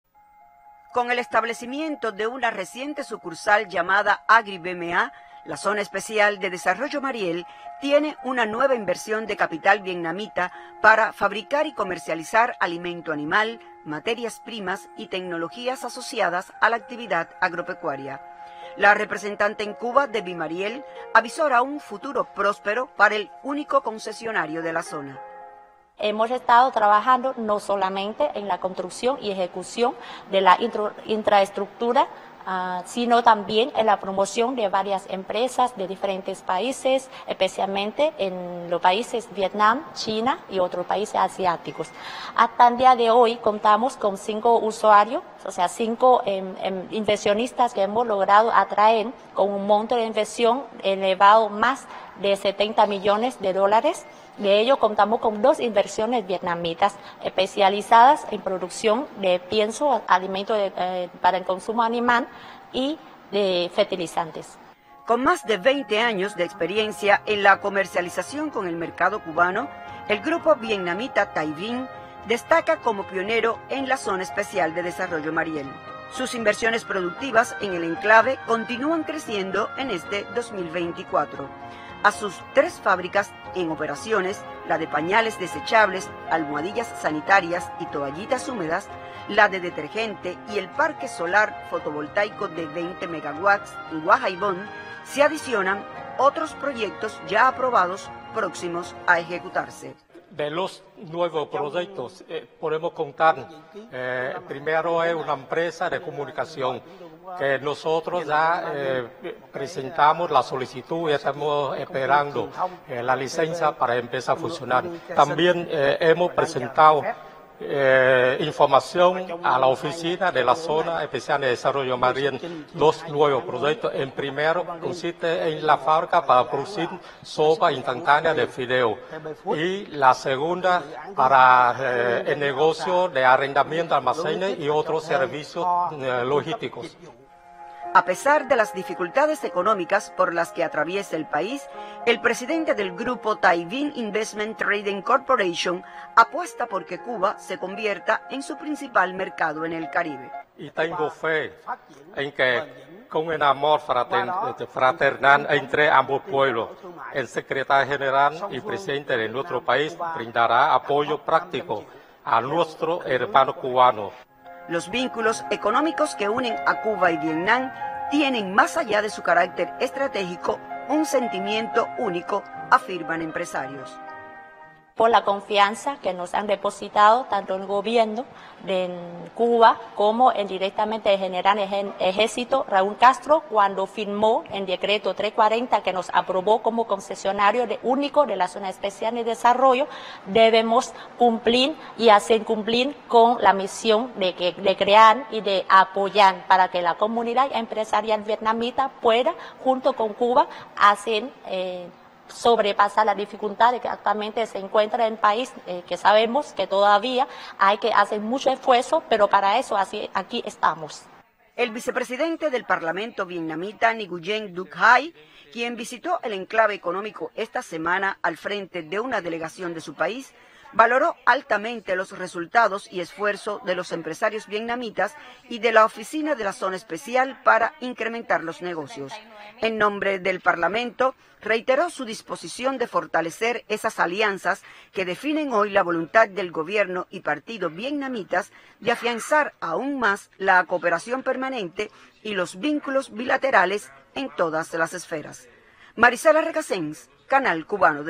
Vietnam principal inversor de Asia y Oceanía en Cuba
Vietnam es el principal inversor de Asia y Oceanía en Cuba, con varios proyectos en diferentes fases, vinculados con la producción de bienes de primera necesidad, energía renovable, materiales de construcción y el sector agroalimentario.